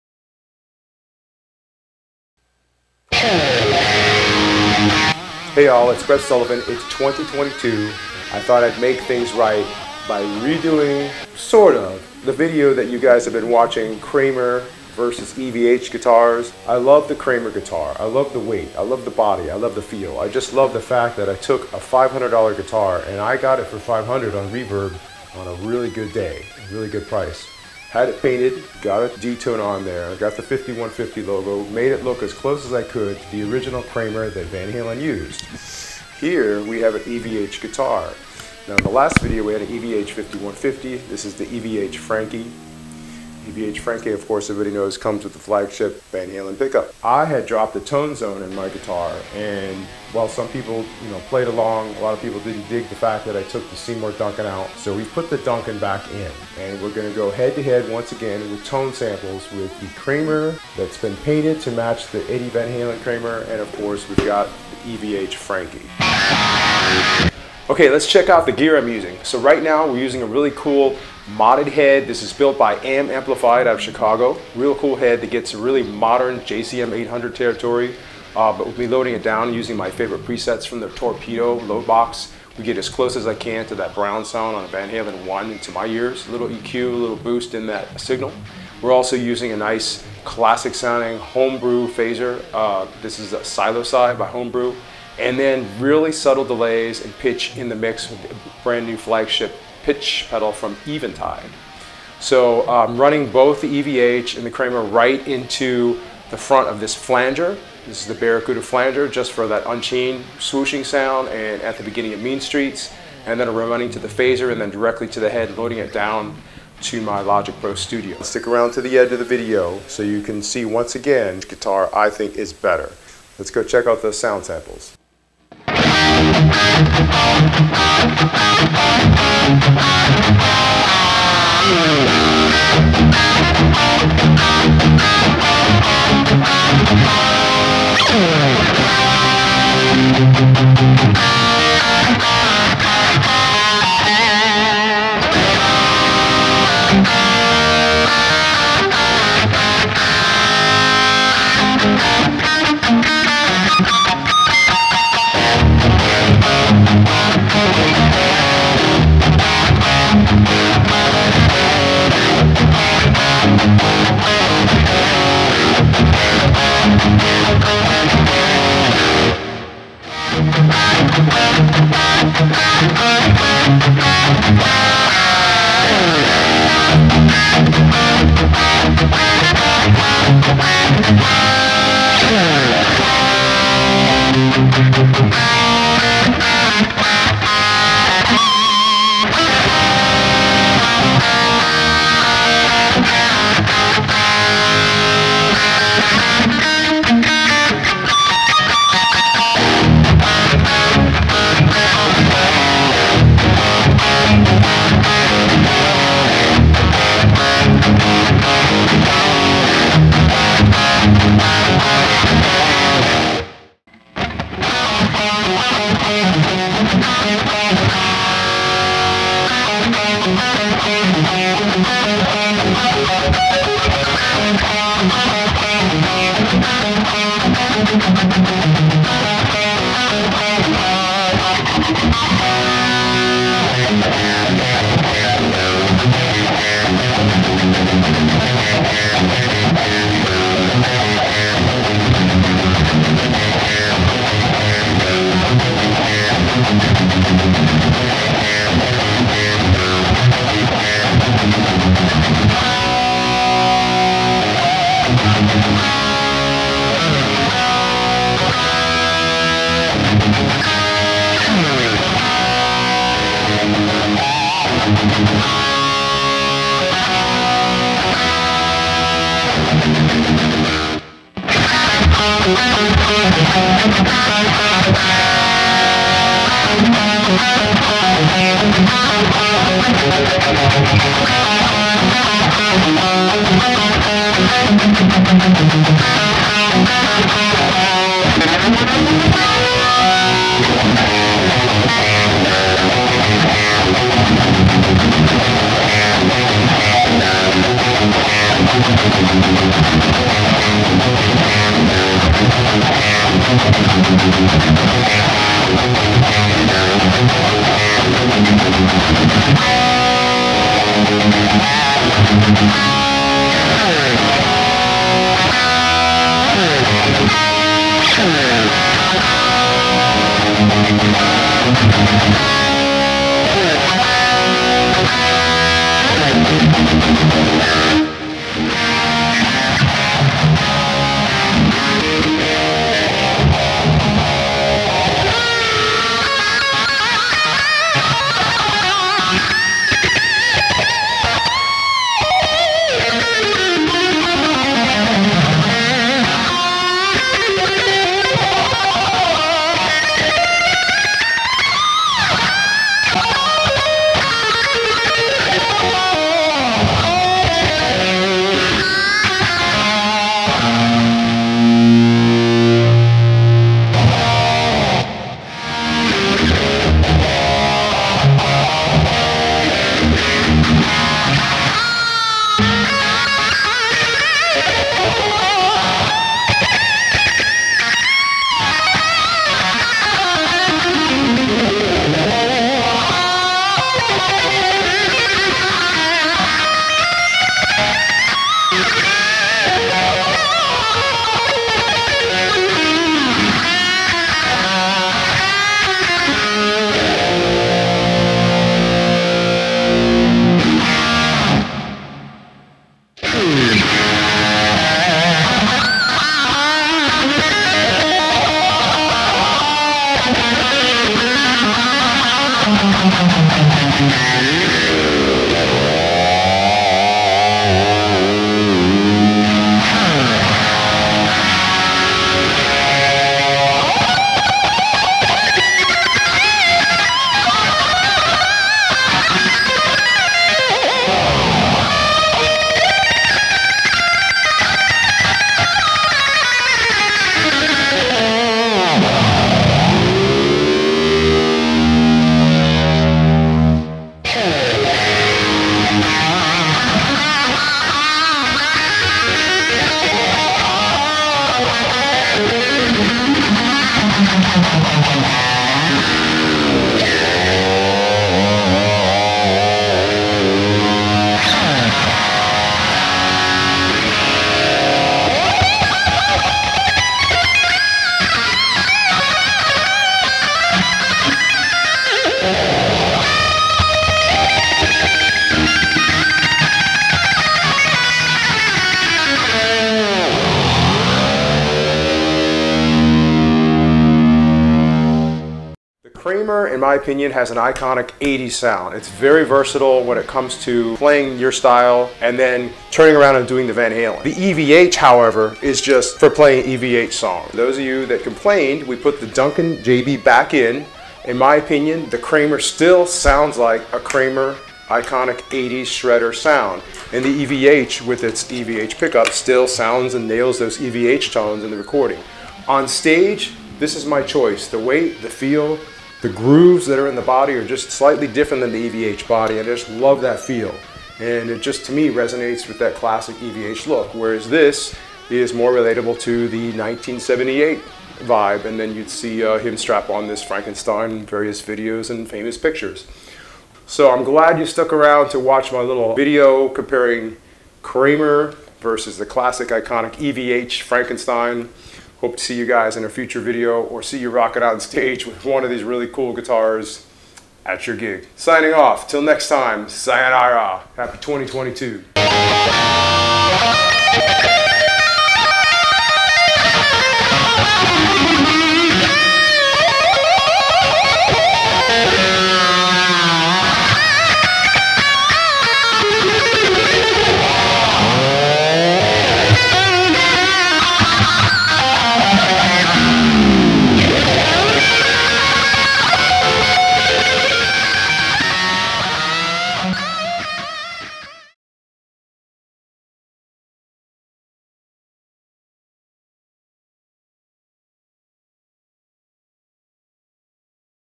Kramer 84 vs EVH 5150- Brev Sullivan
A part two follow up to my original video comparing both the Evh 5150 and the EVH Frankenstrat.